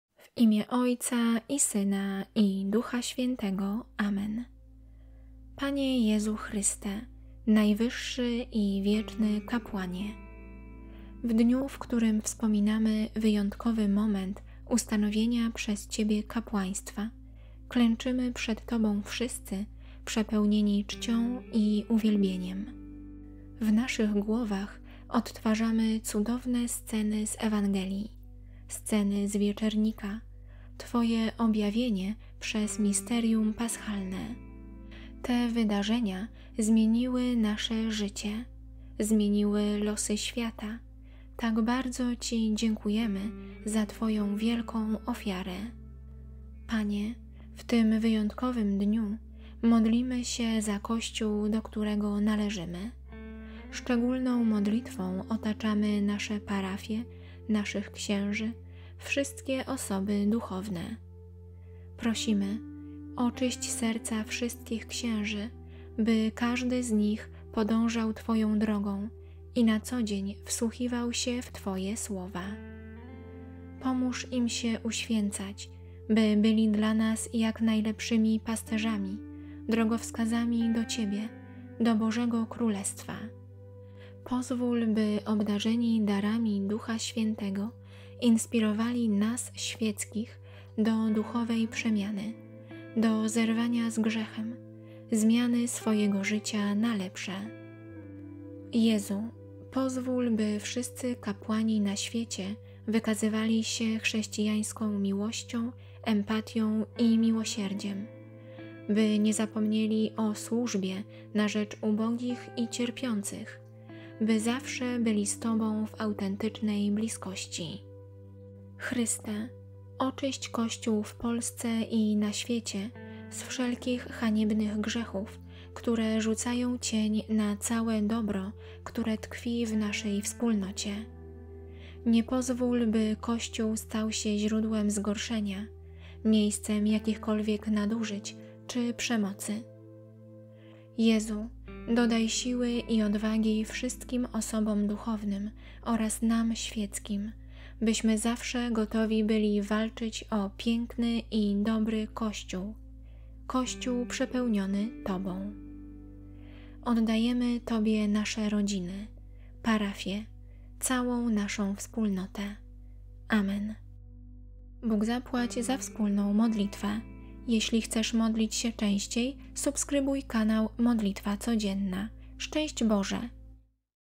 MODLITWA DO CHRYSTUSA NAJWYŻSZEGO KAPŁANA | Modlitwa na święto Chrystusa
MODLITWA DO CHRYSTUSA NAJWYŻSZEGO KAPŁANA to okazjonalne modlitwa przeznaczona do prywatnego odmawiania za cały Kościół, w szczególności za osoby duchowne.

Panie Jezu Chryste, Najwyższy i Wieczny Kapłanie! W dniu, w którym wspominamy wyjątkowy moment ustanowienia przez Ciebie kapłaństwa, klęczymy przed Tobą wszyscy przepełnieni czcią i uwielbieniem. W naszych głowach odtwarzamy cudowne sceny z Ewangelii, sceny z Wieczernika, Twoje objawienie przez Misterium Paschalne. Te wydarzenia zmieniły nasze życie, zmieniły losy świata - tak bardzo Ci dziękujemy za Twoją wielką ofiarę.
Panie! W tym wyjątkowym dniu modlimy się za Kościół, do którego należymy. Szczególną modlitwą otaczamy nasze parafie, naszych księży, wszystkie osoby duchowne. Prosimy, oczyść serca wszystkich księży, by każdy z nich podążał Twoją drogą i na co dzień wsłuchiwał się w Twoje słowa.  Pomóż im się uświęcać, by byli dla nas jak najlepszymi Pasterzami, drogowskazami do Ciebie, do Bożego Królestwa. Pozwól, by obdarzeni darami Ducha Świętego, inspirowali nas, świeckich, do duchowej przemiany, do zerwania z grzechem, zmiany swojego życia na lepsze.
Jezu, pozwól, by wszyscy kapłani na świecie wykazywali się chrześcijańską miłością, empatią i miłosierdziem, by nie zapomnieli o służbie na rzecz ubogich i cierpiących, by zawsze byli z Tobą w autentycznej bliskości.
Chryste, oczyść Kościół w Polsce i na świecie z wszelkich haniebnych grzechów, które rzucają cień na całe dobro, które tkwi w naszej wspólnocie. Nie pozwól, by Kościół stał się źródłem zgorszenia, miejscem jakichkolwiek nadużyć czy przemocy.
Jezu! Dodaj siły i odwagi wszystkim osobom duchownym oraz nam, świeckim, byśmy zawsze gotowi byli walczyć o piękny i dobry Kościół, Kościół przepełniony Tobą. Oddajemy Tobie nasze rodziny, parafie, całą naszą wspólnotę. Amen.